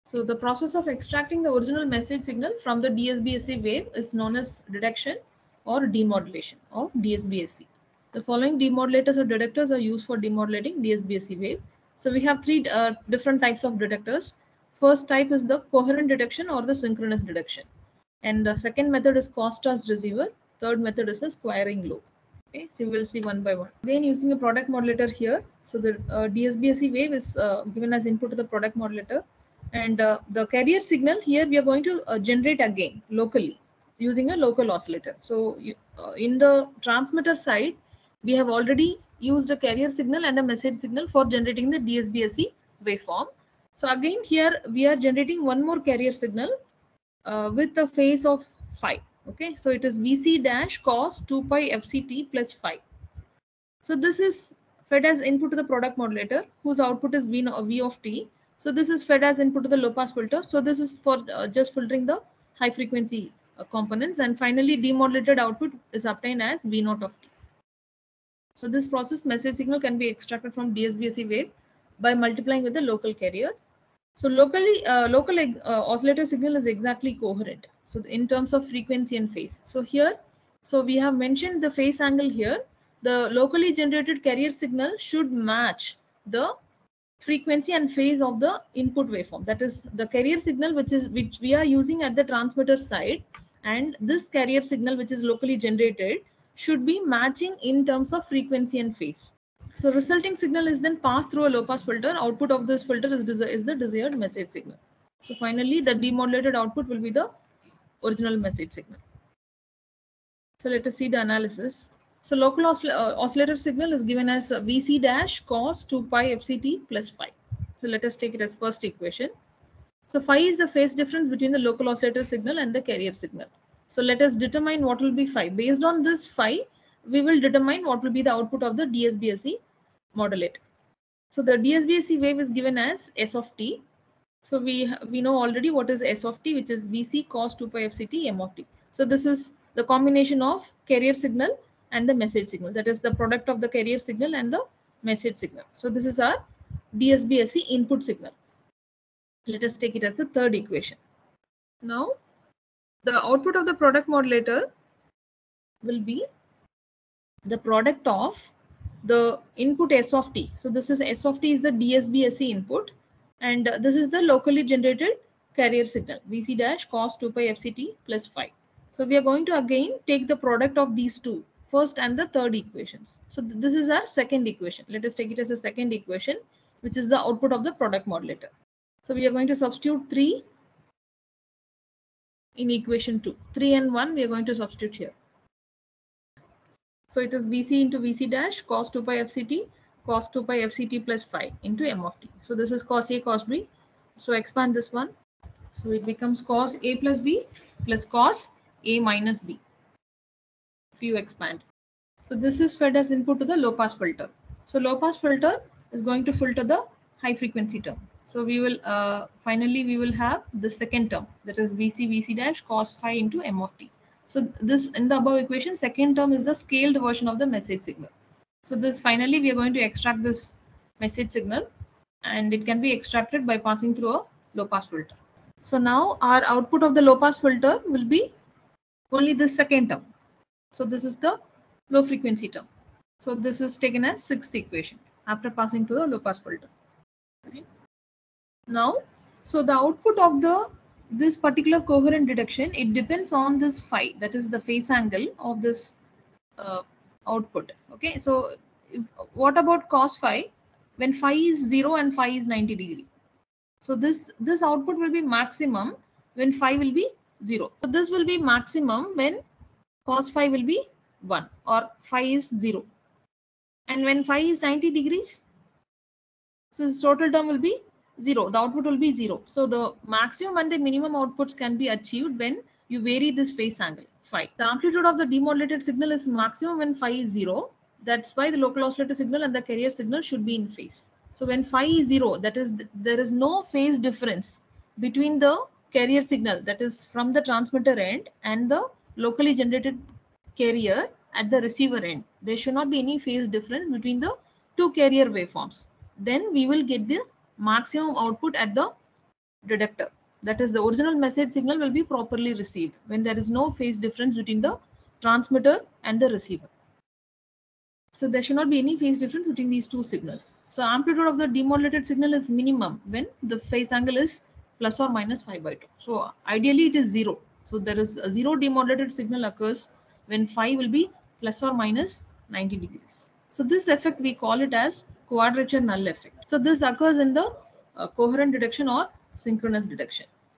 DSBSC Coherent detection
In this video lecture we discuss the Double side band suppressed carrier AM detection using Coherent detection process.